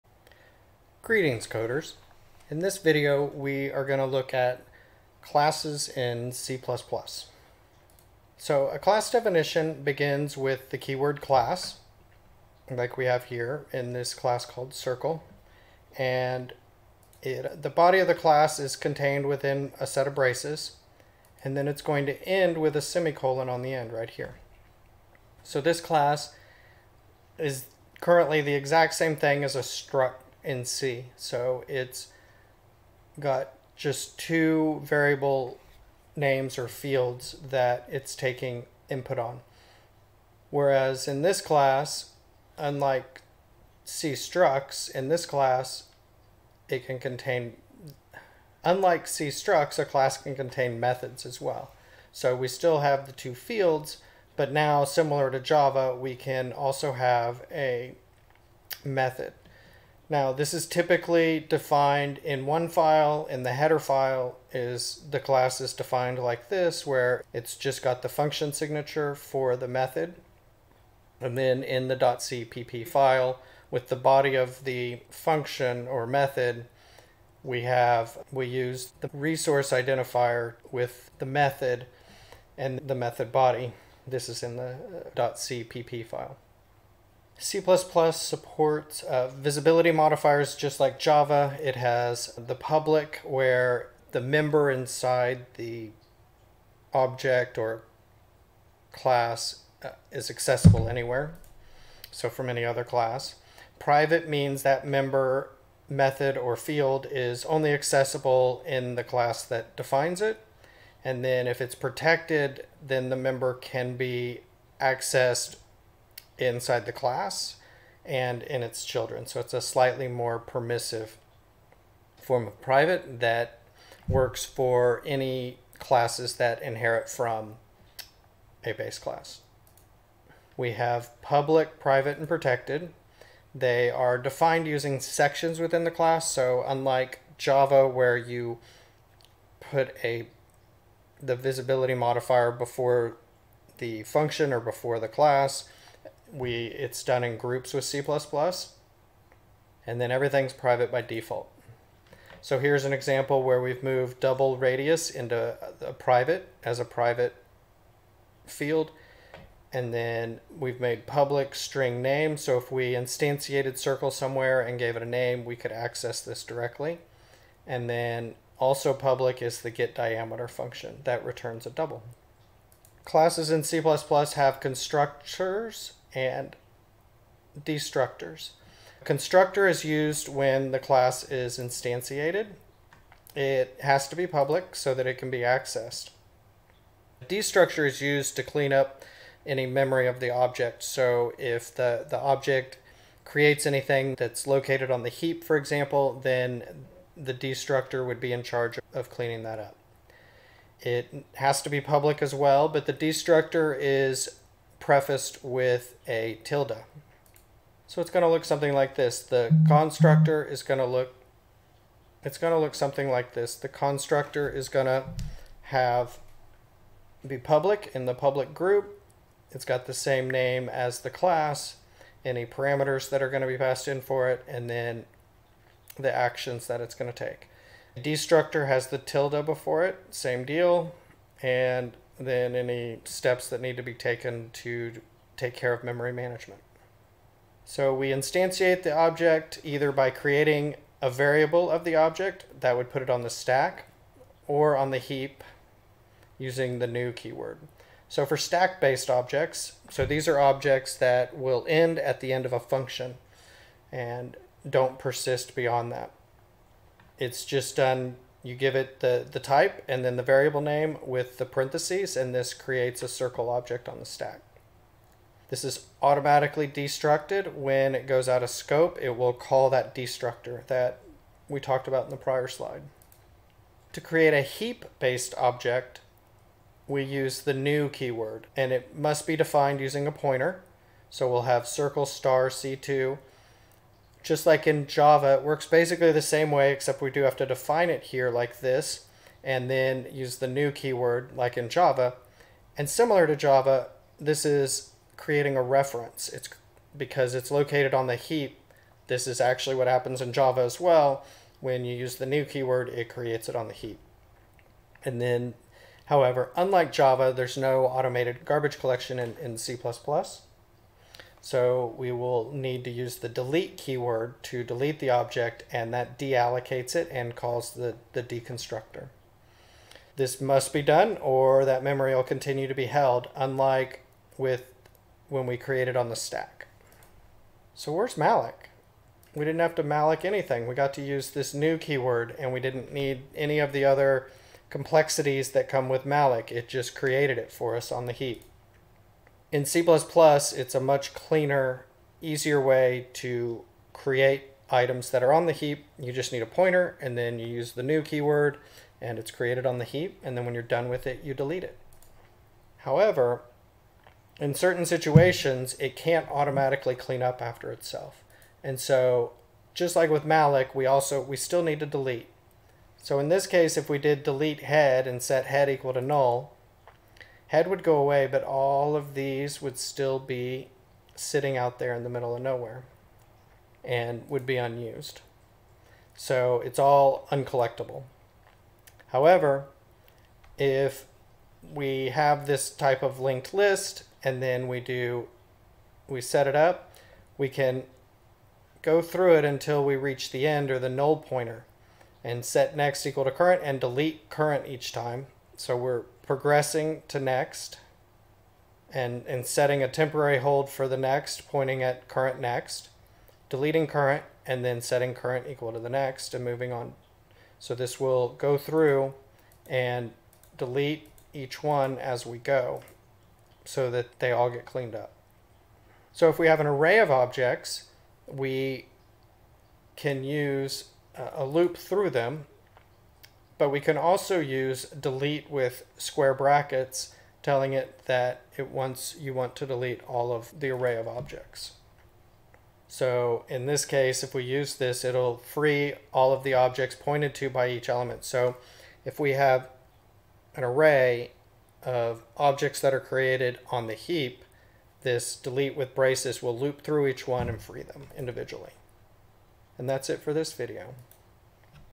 3.1.3.0 C++ Classes
This video covers the use of classes in C++.